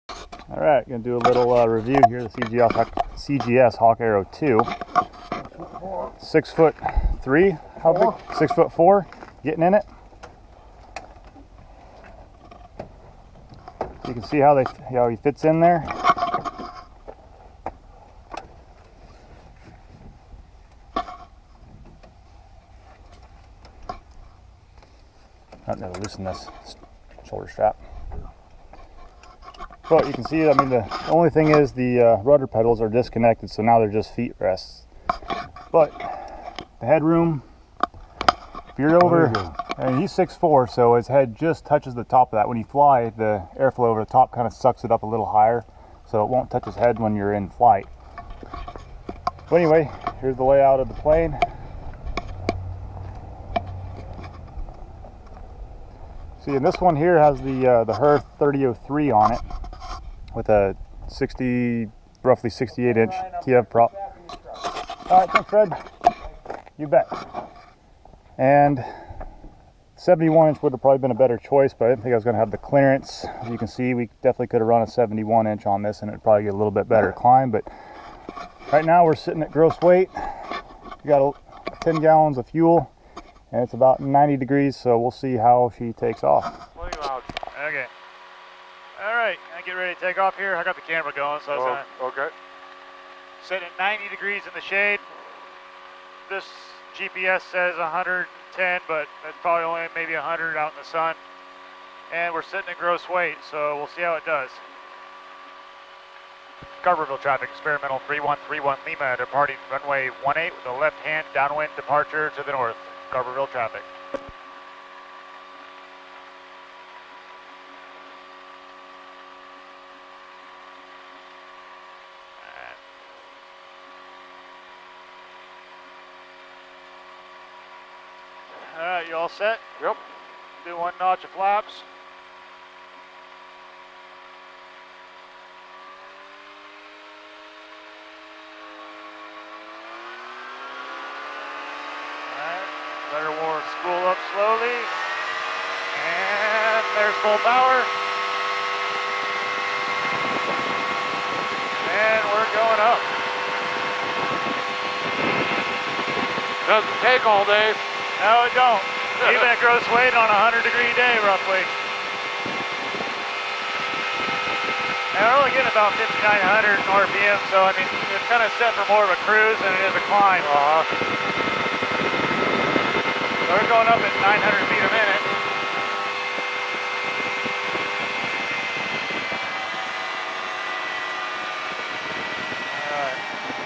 CGS Hawk Passenger Loading
takeoff at gross weight on a 100 deg F day.